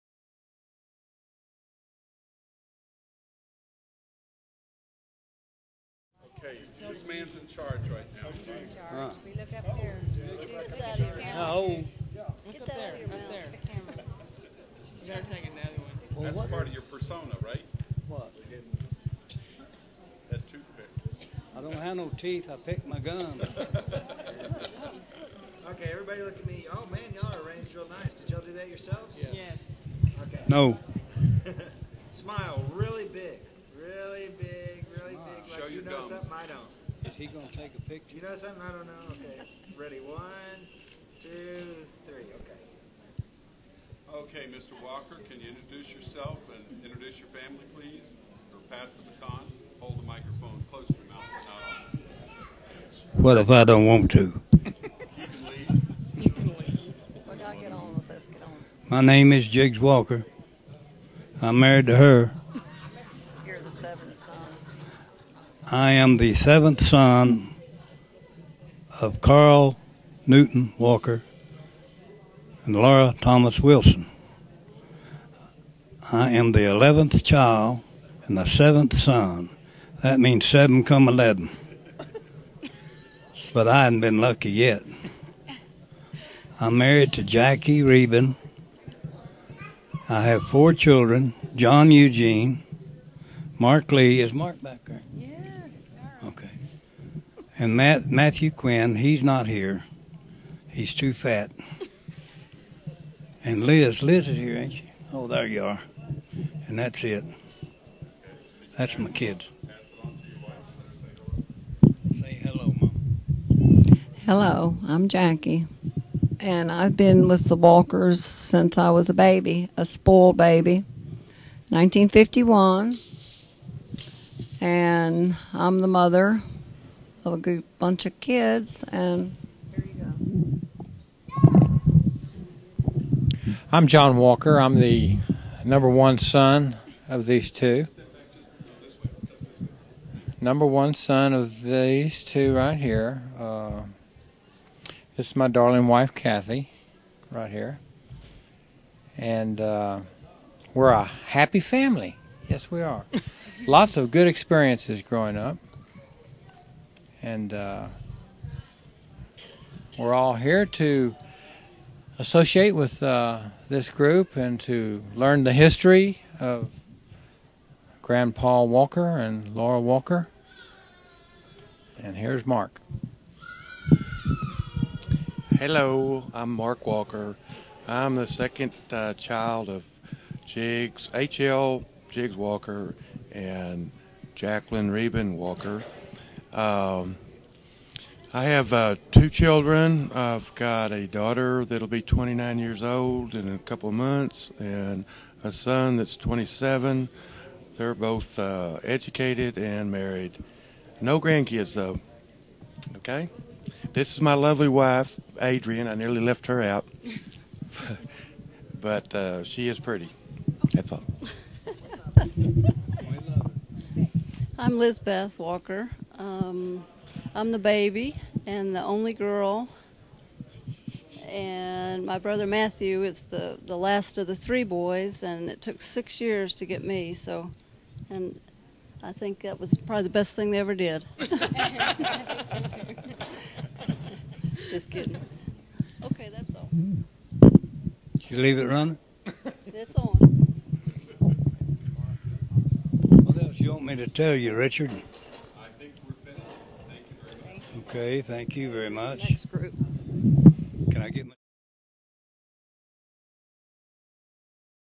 04 Harold Lee (Jiggs) Walker Family Introductions
The 2008  Carl Newton Walker and Laura Thomas Wilson Walker Family Reunion.

In the summer of 2008, thirty-five Walker descendants, gathered in the atrium of the Lubbock Best Western, Lubbock, Tx.  Five of the  fourteen children and about thirty of their grand children attended.  We had two missions:   1. Eat  and 2. Gab.

After lunch, we took photos and videos of each family.  The video family inventories are scatter among these family stories.Story Time, following the family photo/video session.  These stories were told by a child or grandchild of Carl and Laura Walker.

Some of these stories were written and left by one of the children, and others were spontaneously presented by the grandchildren.

Shot in high definition (HD), these images were compressed when uploaded to the internet.  Since they were subsequently downloaded from the Internet, they lack their full resolution.  The audio is excellent, they are best watched  on small screens.  A few of these videos became dark when a cloud blocked
 the sun.

Richard A. Walker, 2020